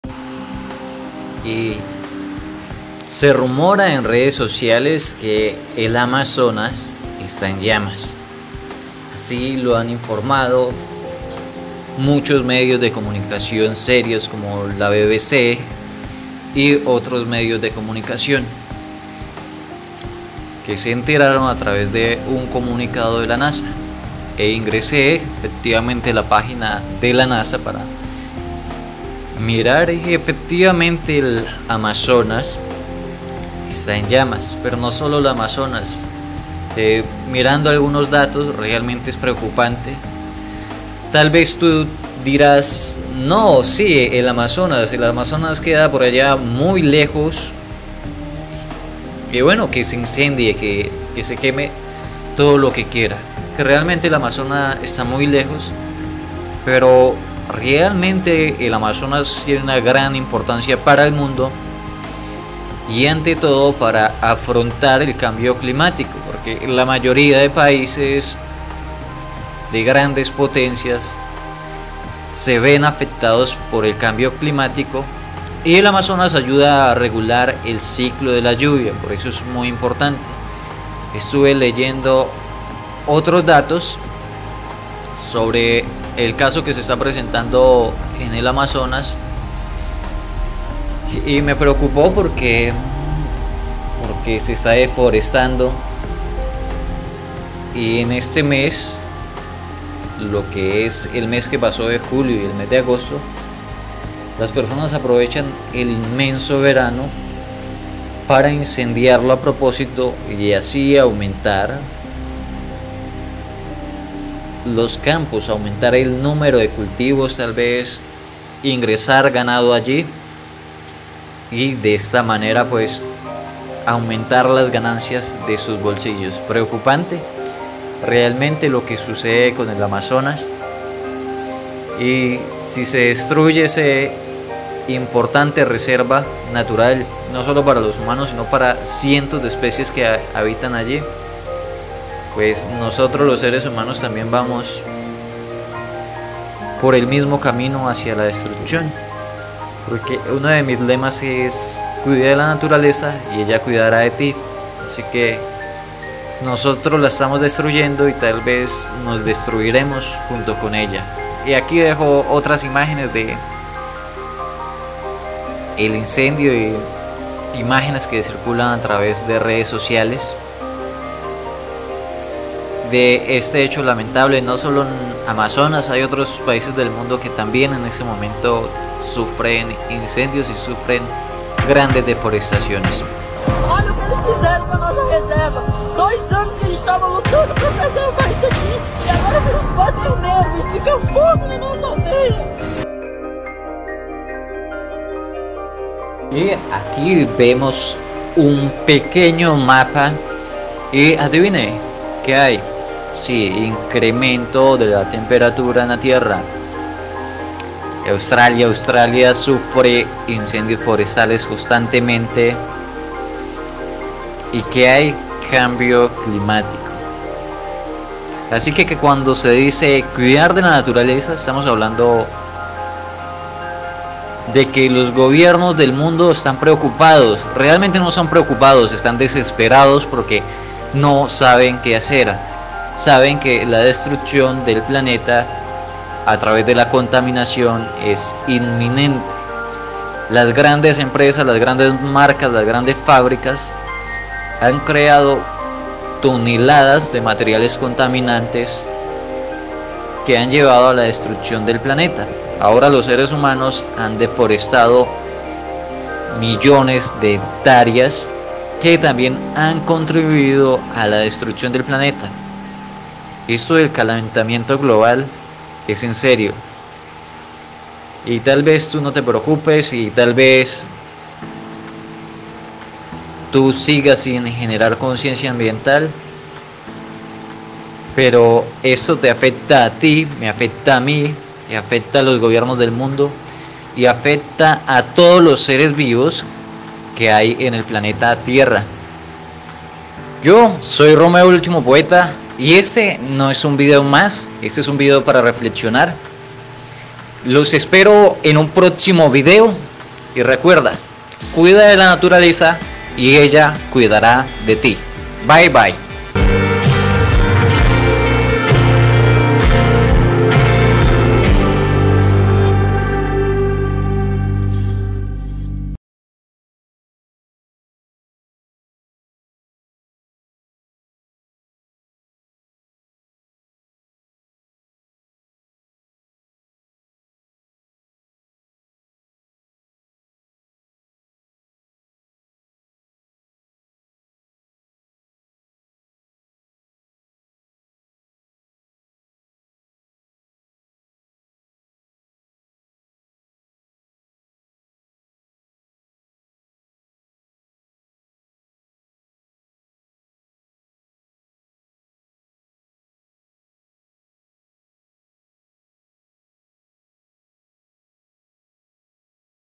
🔥 INCENDIO EN EL AMAZONAS 🔥 INCENDIO EN AMAZONAS ES VERDAD
🔥🔥 ¡ INCENDIO FORESTAL AMAZONAS   ! 🔥🔥
El amazonas arde en llamas en este video les explicare, las implicaciones y la importancia del amazonas para el mundo entero.